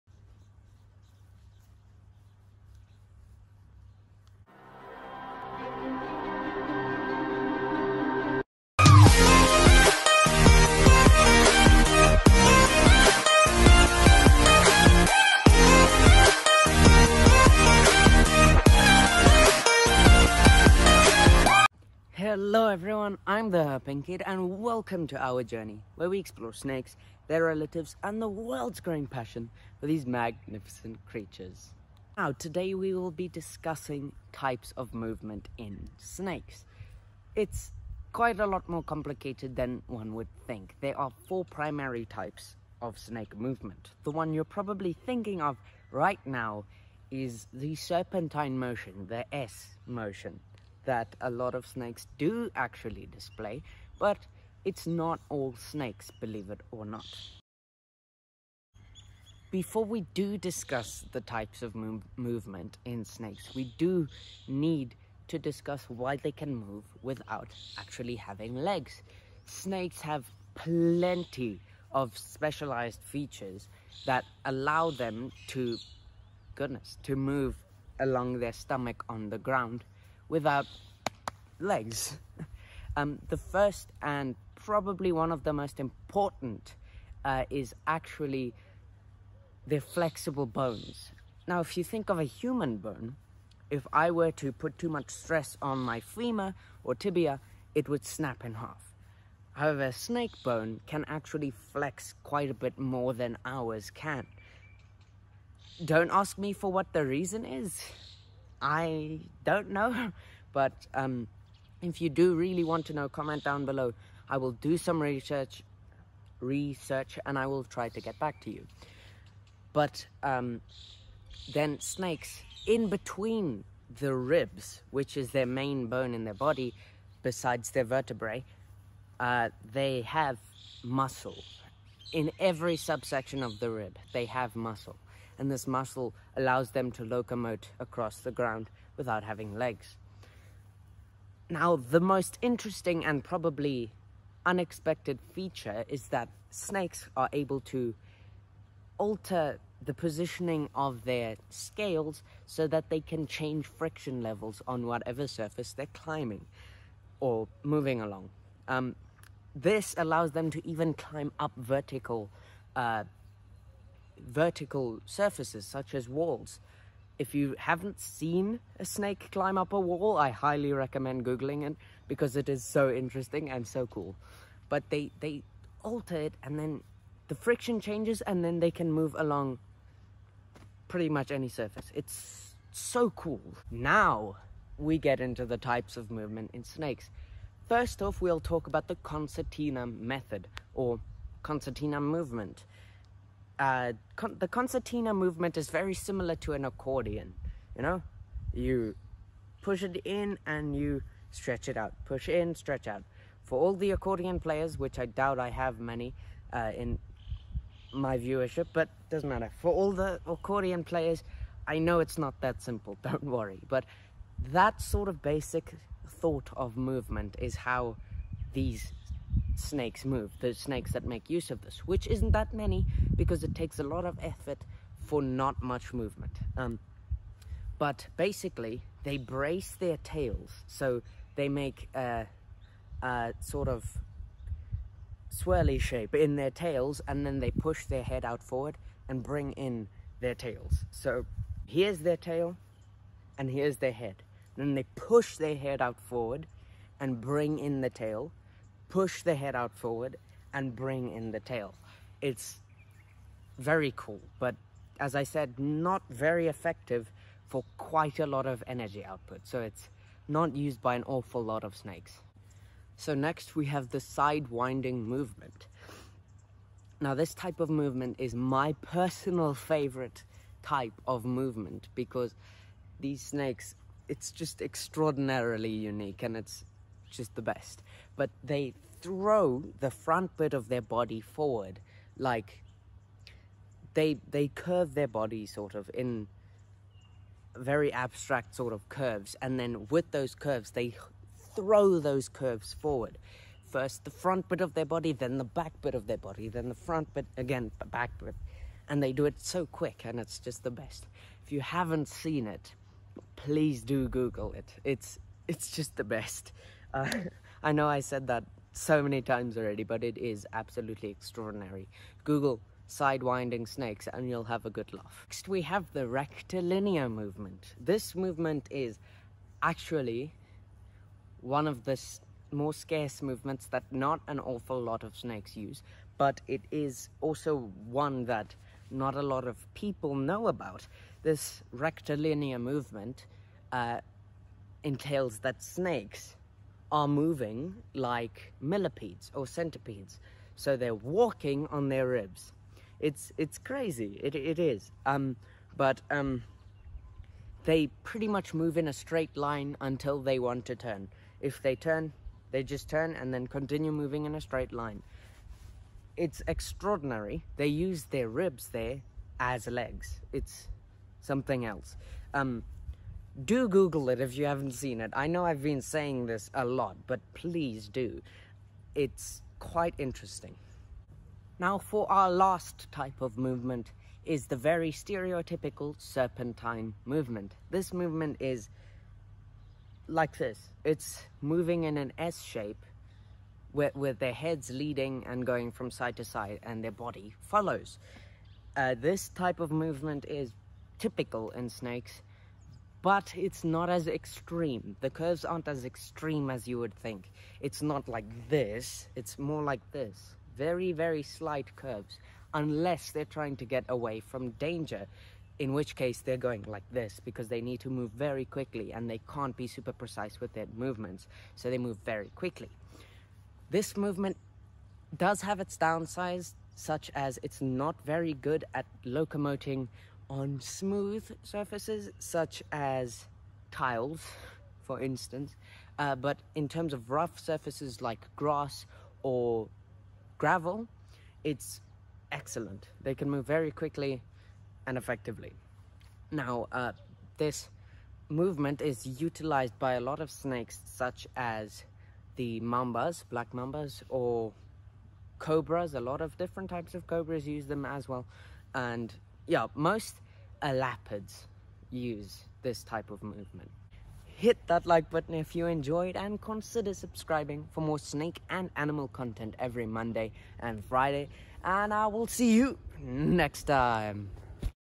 Types of Snake Movement!
Welcome Snake Lovers!
Today we’ll be discussing the types of movement in Snakes!

The AEI website!

The Association of Environmental Issues Instagram!

🕊️Consider following my twitter!

out the DnD Community Server!!